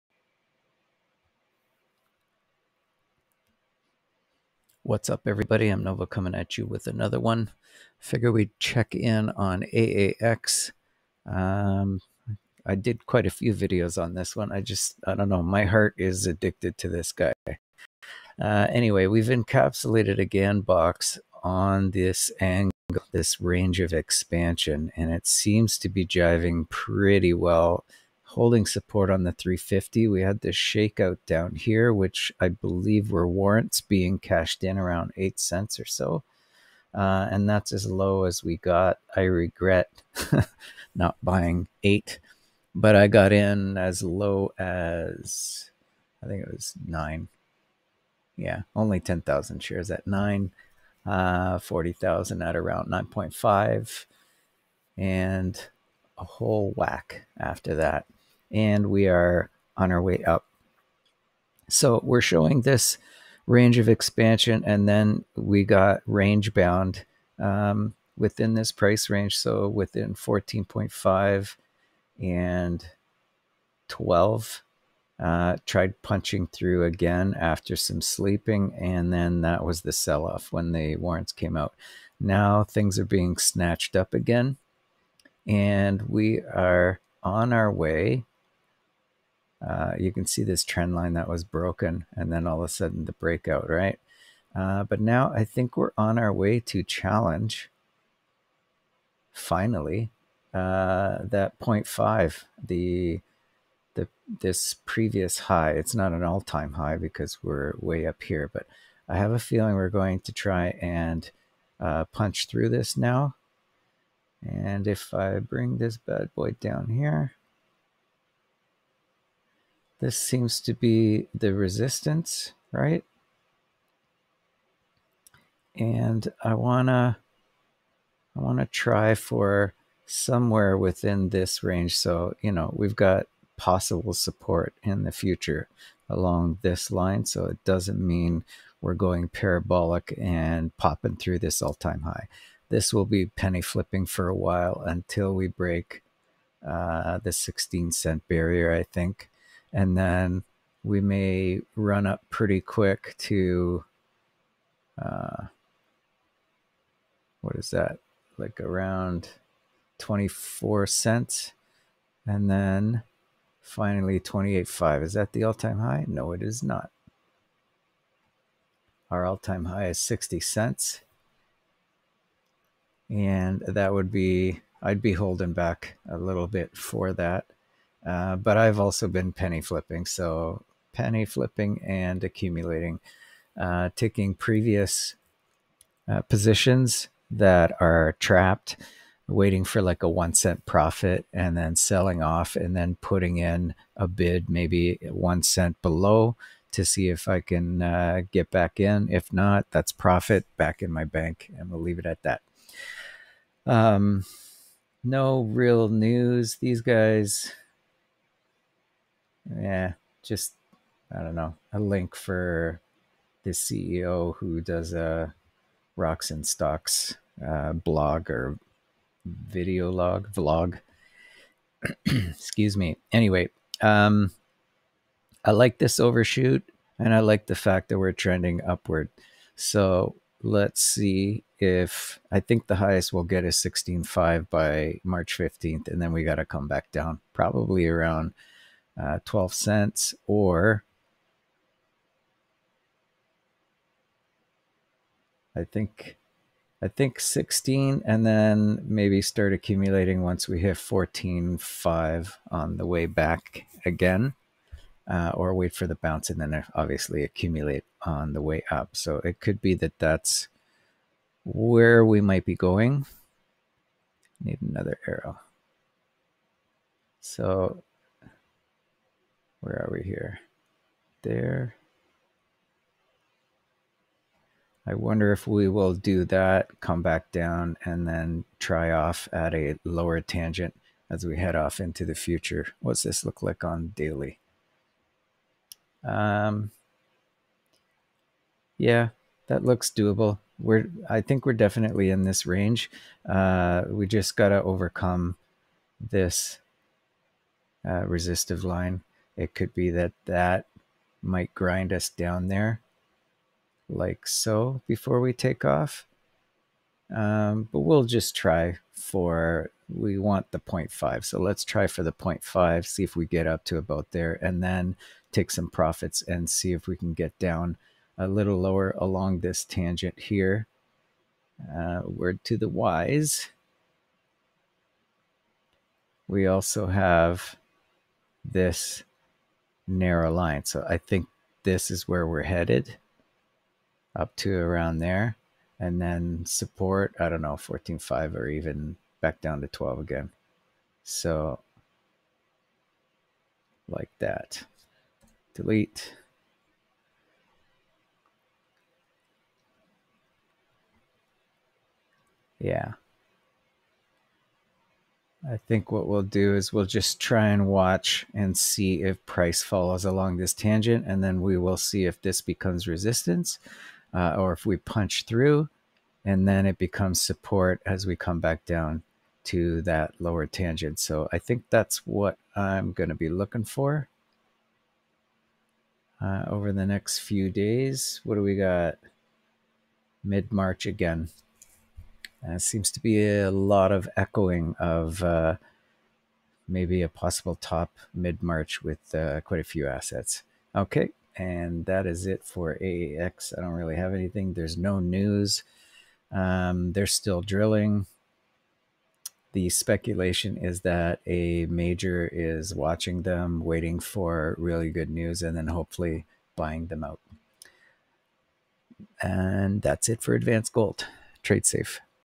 AAX Advanced Gold
AAX weekly looks good. we should finally challenge .16 but I don't think we will break through yet.